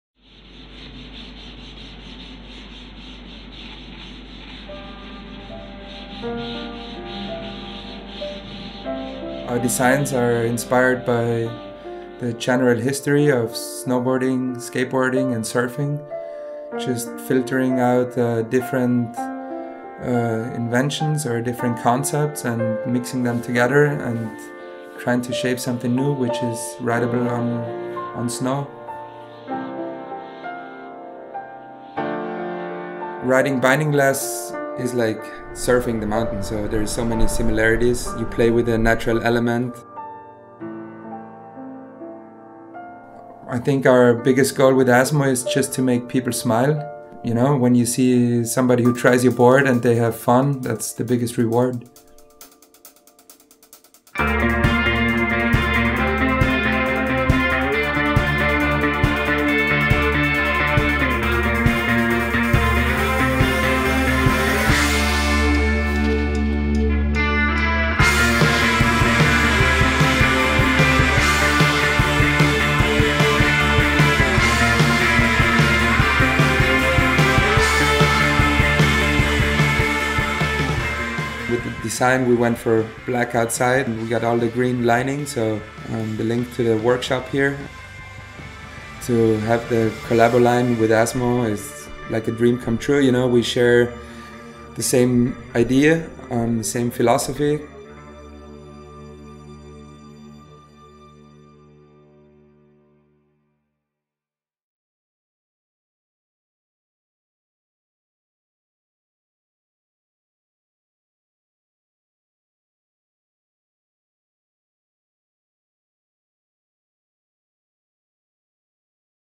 Dakine Aesmo - Die neue Kollektion, inspiert vom "Dakine Team Member" Wolle Nyvelt
Dakine hat sich mit Wolle Nyvelt's Aesmo Pow Surfer, der auch der Erfinder des Noboards ist, zusammen geschlossen. Daraus ist die Kollektion Aesmo entstanden die nicht nur bomensicher aussieht sondern auch so konzipiert ist, dass er länger widerstandsfähiger gegen Einrisse, Abschürfungen und Abnutzung bleibt.